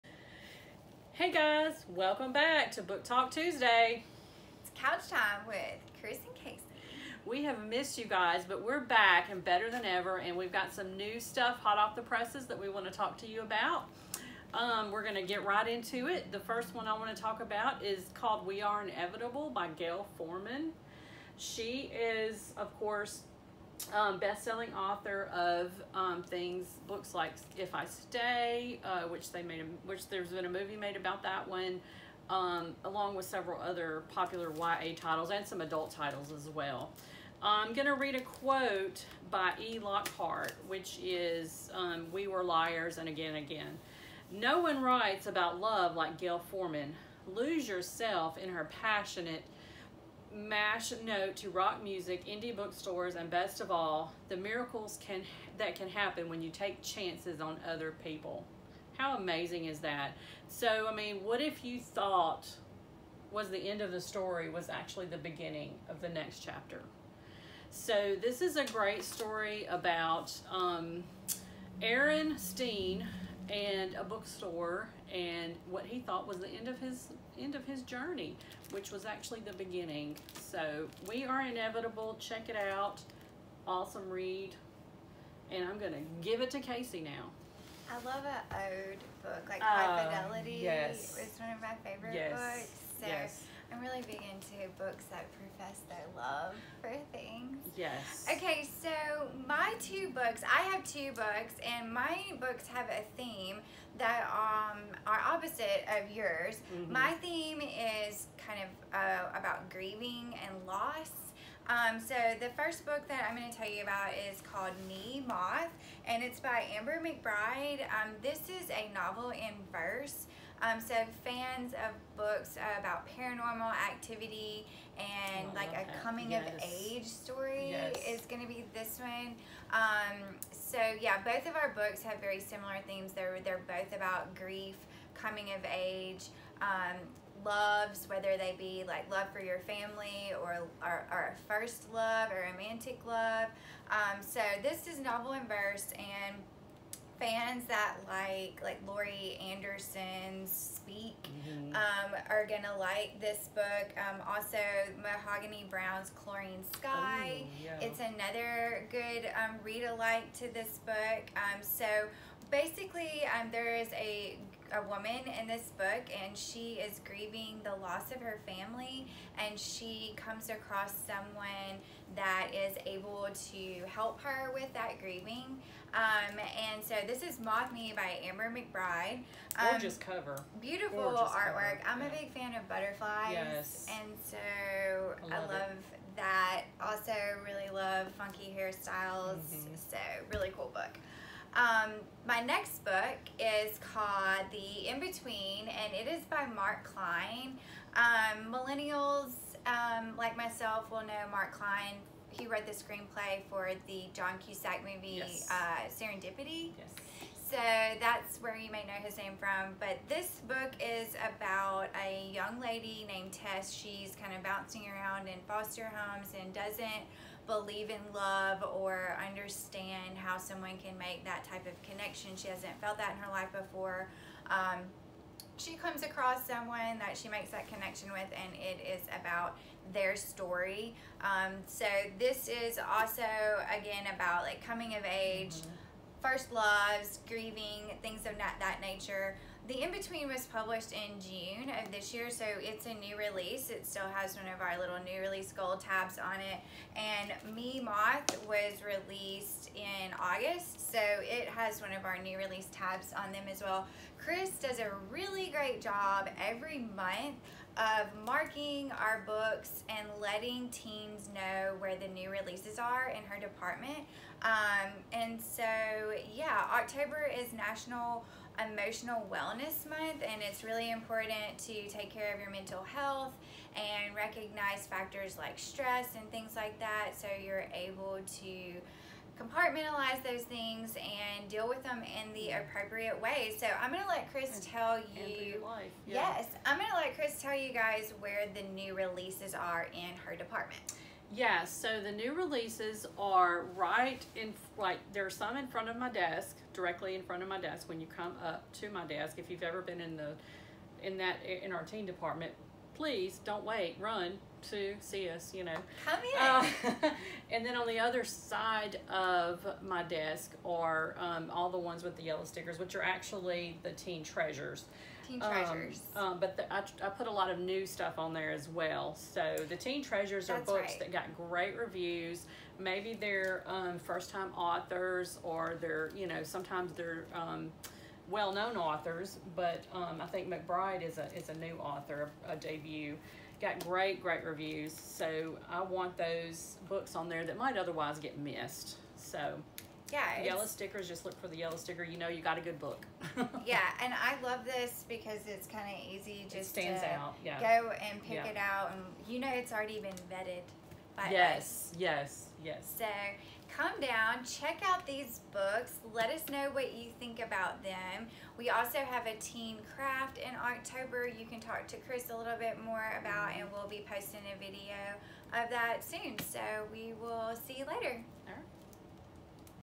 Book Talk Tuesday
Kris and Kacey are back with fresh, new YA releases they can't wait to tell you about. It is finally fall reading weather. Stop by the Teen Dept. and ask us about the new books we have here and what's on the way!